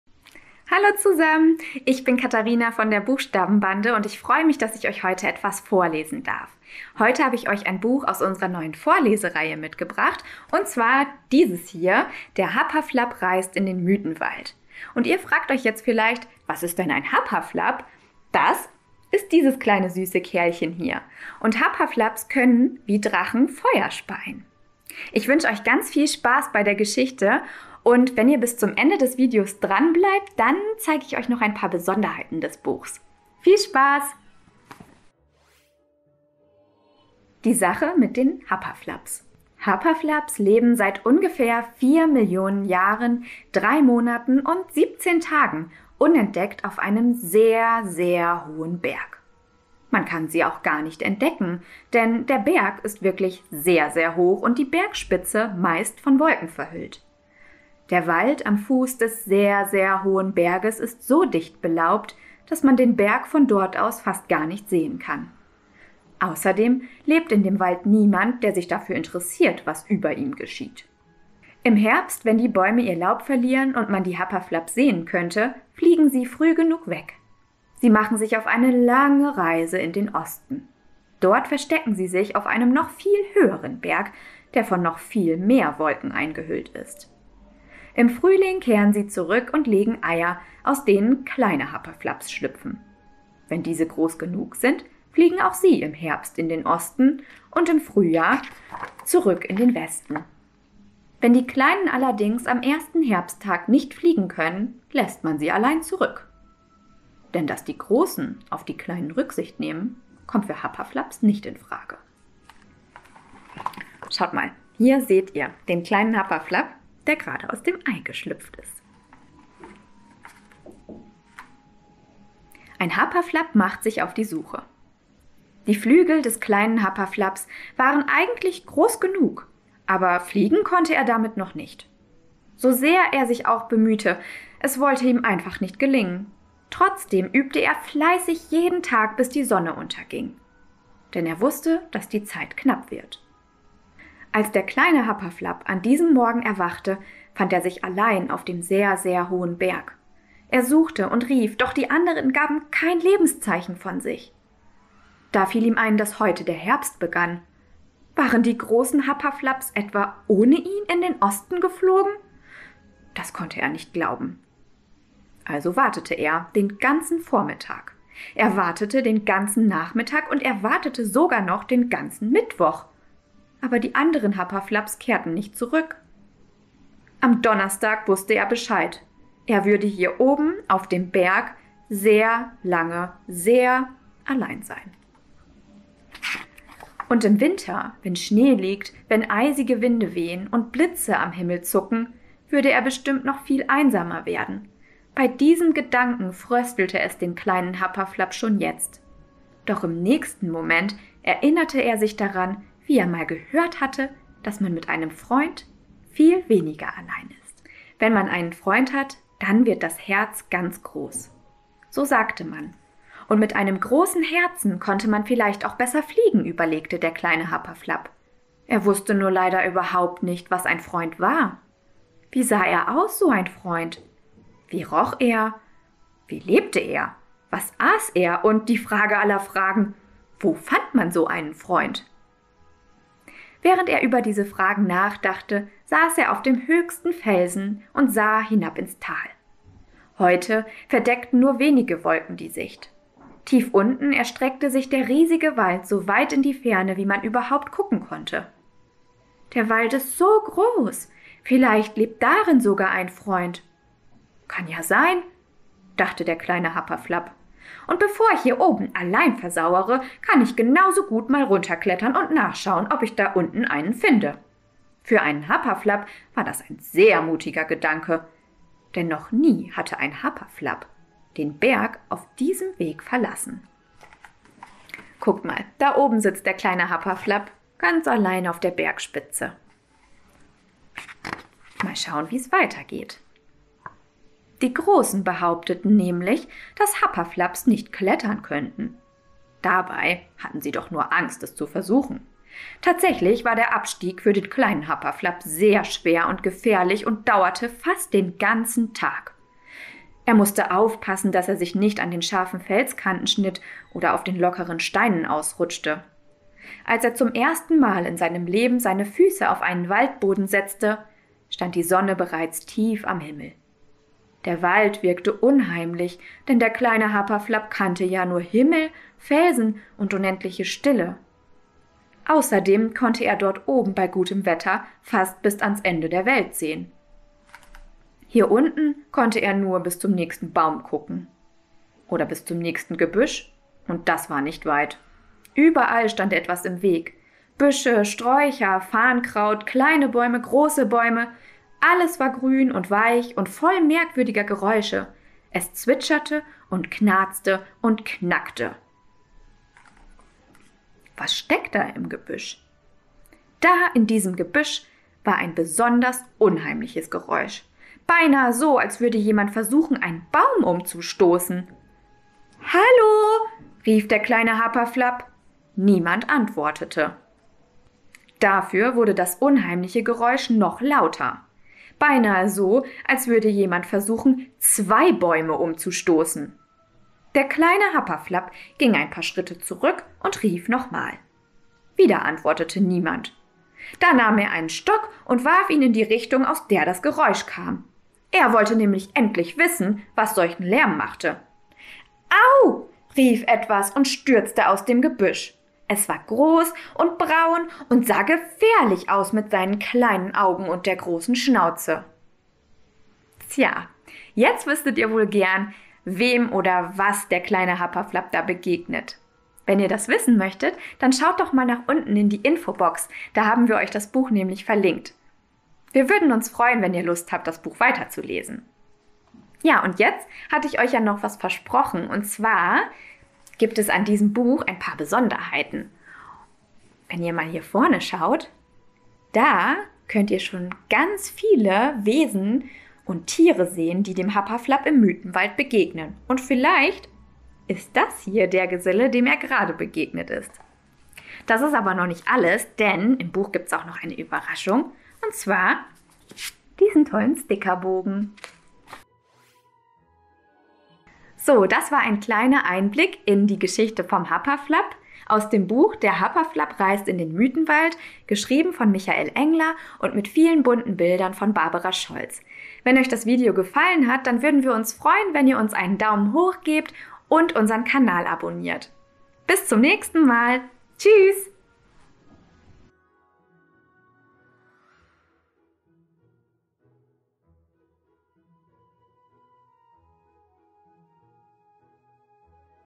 Vorlesevideo: Der Happaflapp reist in den Müthenwald | Engler & Scholz | BuchstabenBande
Vorlesevideo | Vorlesebuch ab 4 Jahren | Der Happaflapp | Vorlesegeschichten | Michael Engler | Barbar Scholz

Katharina von der BuchstabenBande liest euch in diesem Vorlesevideo das Vorlesebuch „Der Happaflapp reist in den Müthenwald“ von MichaelEngler und illustriert von Barbara Scholz vor. Außerdem verrät sie euch ein Geheimnis, denn das Buch aus unsere neuen Reihe VORLESEN! hält jede Menge Überraschungen für euch bereit 😉

Darum geht’s: Happaflapps leben seit ungefähr vier Millionen Jahren, drei Monaten und siebzehn Tagen unentdeckt auf einem sehr, sehr hohen Berg. Wie Drachen können sie Feuer speien und im Herbst fliegen sie in den Osten. Vorausgesetzt, man kann fliegen. Genau darin aber liegt das Problem des kleinen Flapps. Und so wacht er eines Morgens auf und ist plötzlich ganz allein. Doch er erinnert sich daran, was er einmal gehört hat: Mit einem Freund ist man angeblich weniger allein. Also wagt sich Flapp von seinem Berg und macht sich auf in den Müthenwald. Denn dort, wo Professohren hausen und Wickelmützen leben, findet sich bestimmt irgendwo auch ein Freund.

Eine zauberhafte Freundschaftsgeschichte, in der es vor lustigen Wesen nur so wimmelt. Einfühlsam erzählt von Michael Engler mit wunderschönen Illustrationen von Barbara Scholz

⭐ Hier erfährst du mehr zu unsere VORLESEN!-Reihe und erhältst Tipps für ein tolles Vorleseerlebnis